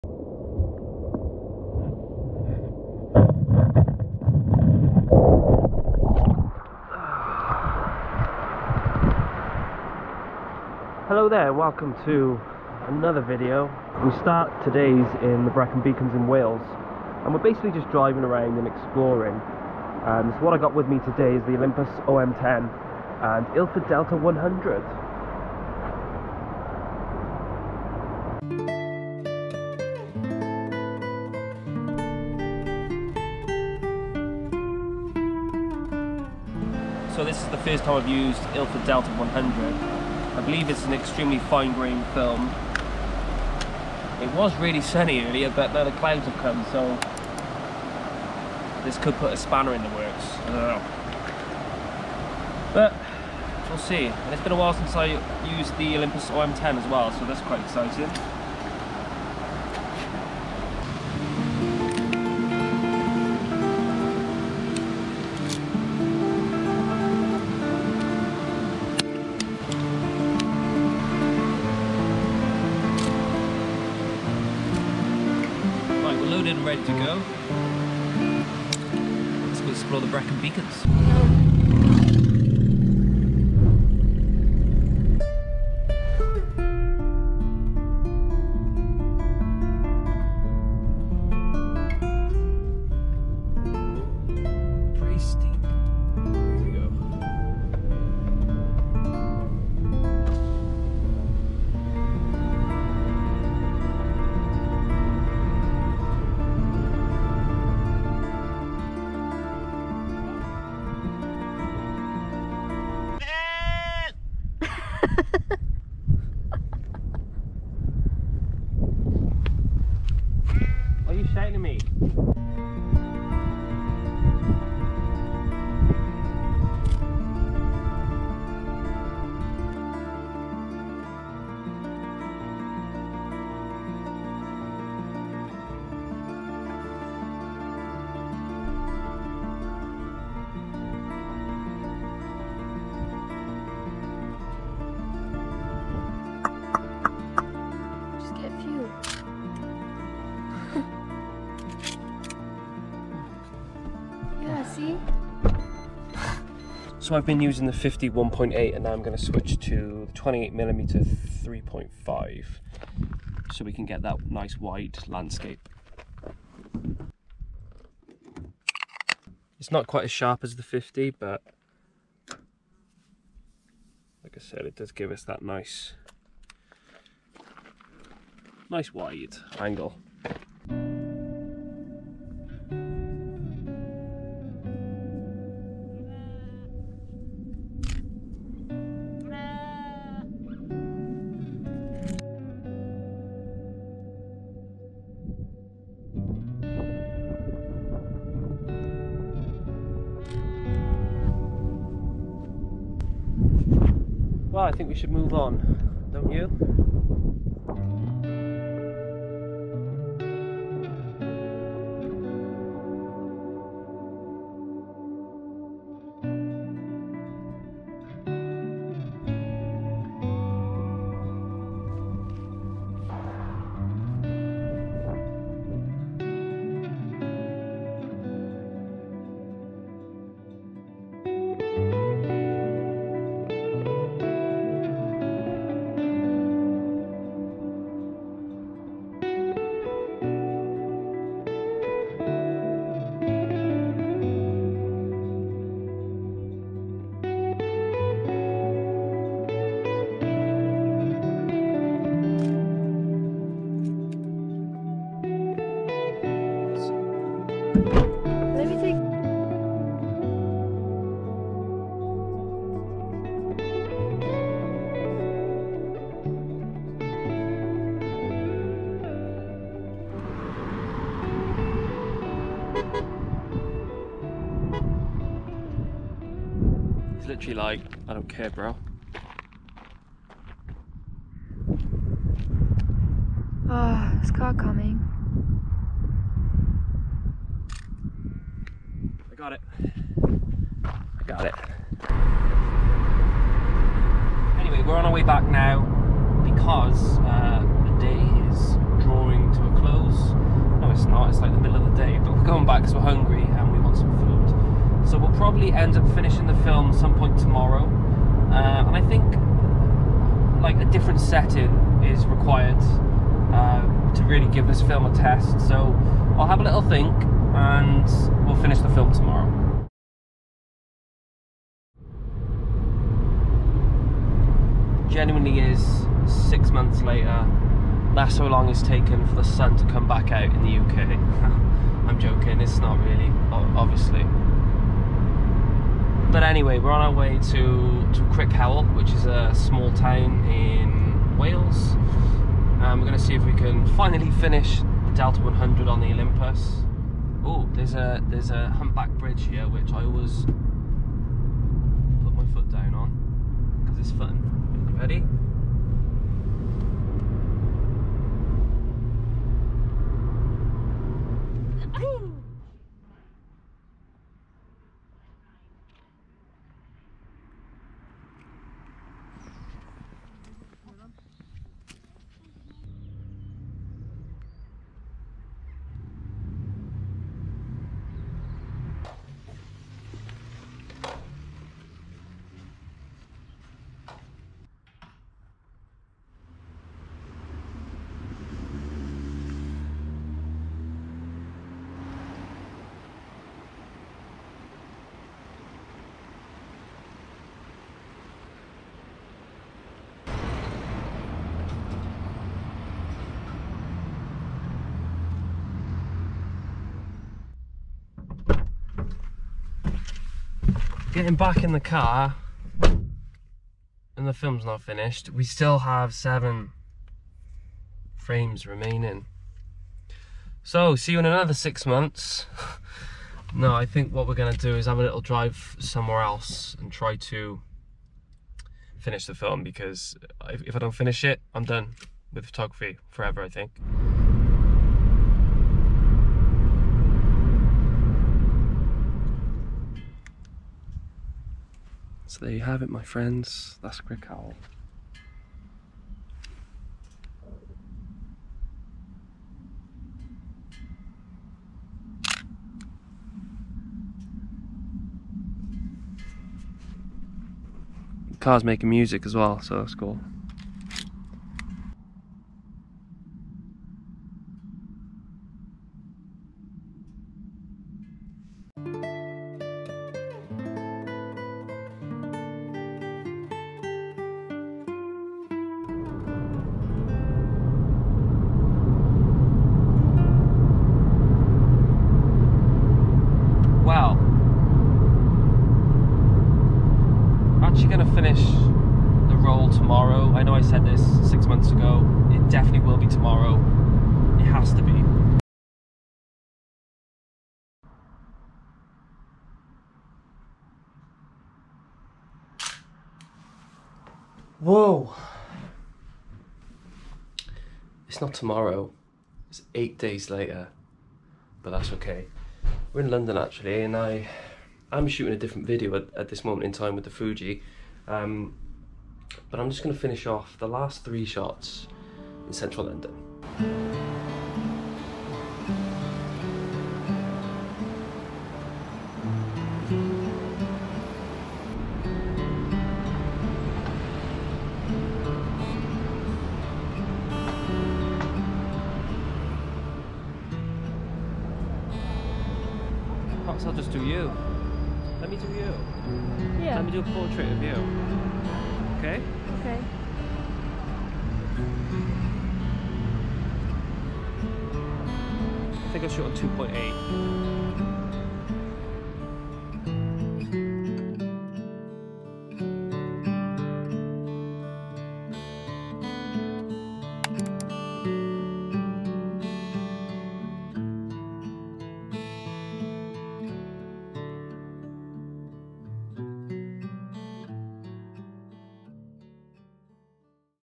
Olympus OM10 with Ilford Delta 100
I take a look at the Olympus OM10 and pair it with Ilford Delta 100 film. This is a great first camera for a beginner in Film/Analogue Photography who wants to use manual settings. Ilford Delta 100 was a little tricky to shoot with but I love this black and white film stock. Overall it was a joy to shoot with the Olympus OM10, I highly recommend other film enthusiasts to give it a try.

In this video, we shoot some landscape film photography in the Brecon Beacons and more images in Crickhowell, Wales and London, England.

The film is not pushed or pulled at all. Shot using the OM10s built in light meter. Developed and scanned at a professional lab.

Chapters
00:00 - 01:20 - Introduction
01:21 - 05:18 - Brecon Beacons
05:19 - 08:14 - Crickhowell
08:15 - 09:23 - London
09:24 - 09:40 - Thanks & Subscribe